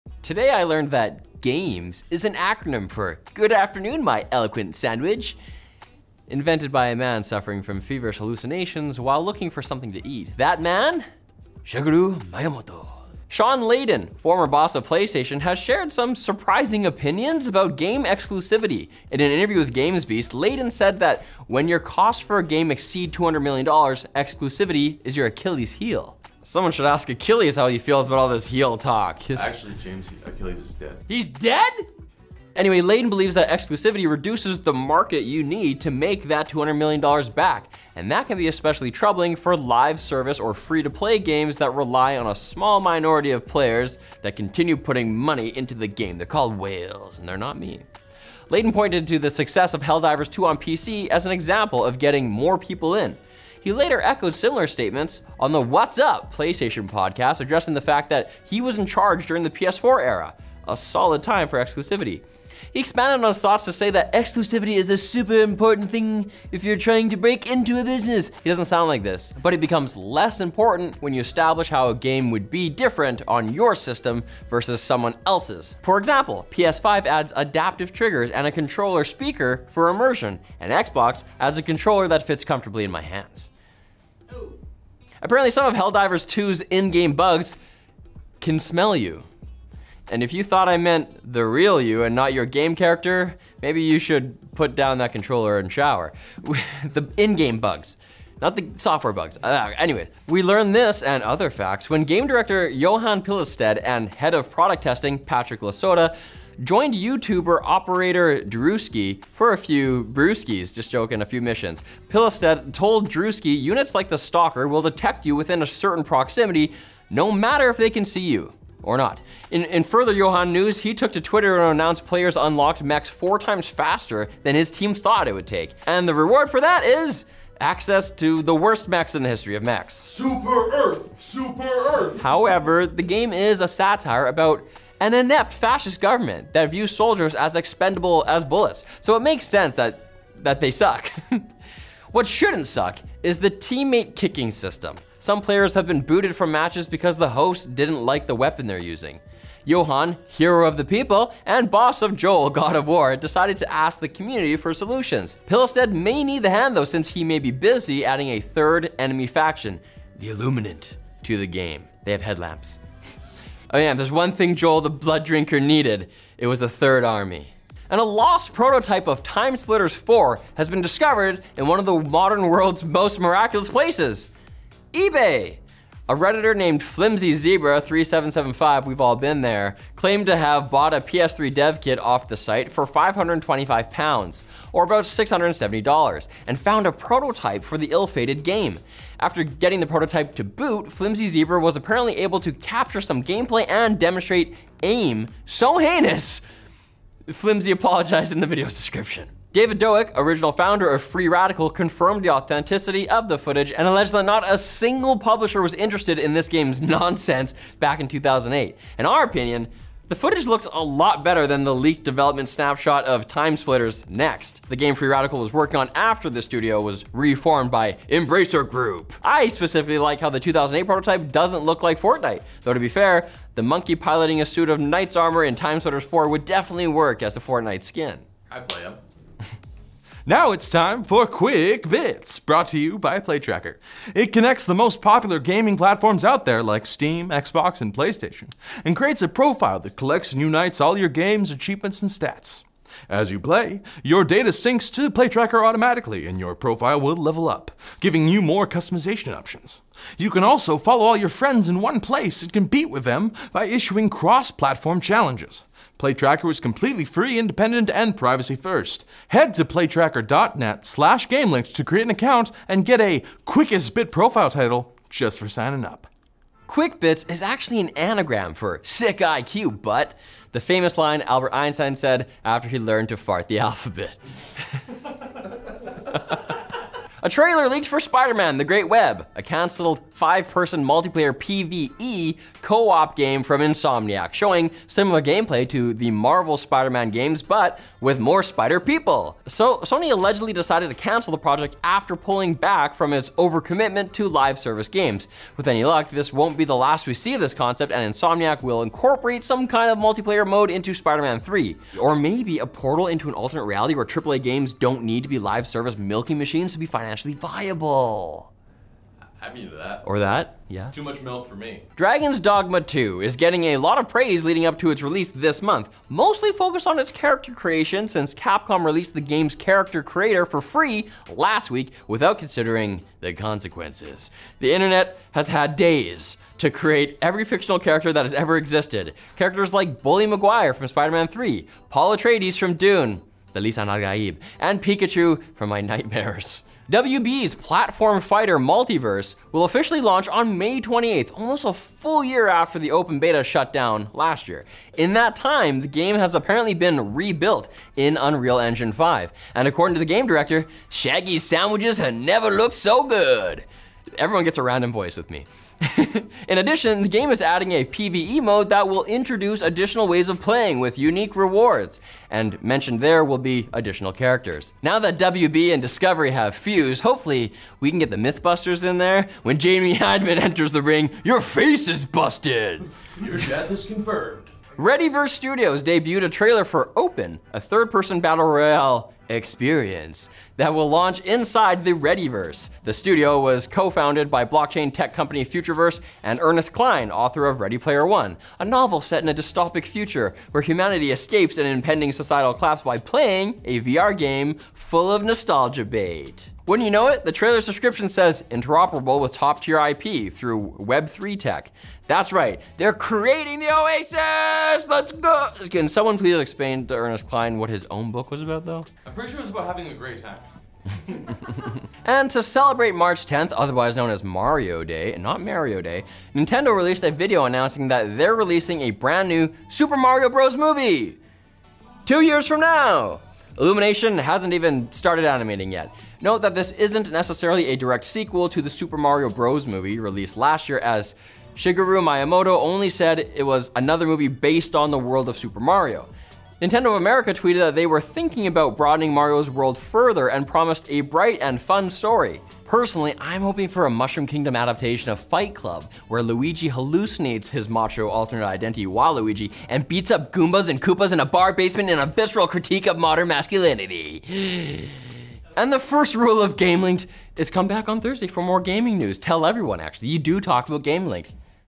Helldivers 2 proves that Exclusives Should End
Achievements don’t need to end when you get that platinum.

Timestamps:
0:00 Acronyms are fun
0:13 Ex-PS boss says exclusives are outdated
1:25 Big Helldivers 2 news round-up
2:47 Lost TimeSplitters 4 prototype found
4:26 QUICK BITS INTRO
4:37  cancelled Co-op Spider-Man game
5:11 Dragon's Dogma 2 character creation storm
5:37 MultiVersus launching May 28
6:14 Ready Player One IRL
6:52 New movie "based on" Mario universe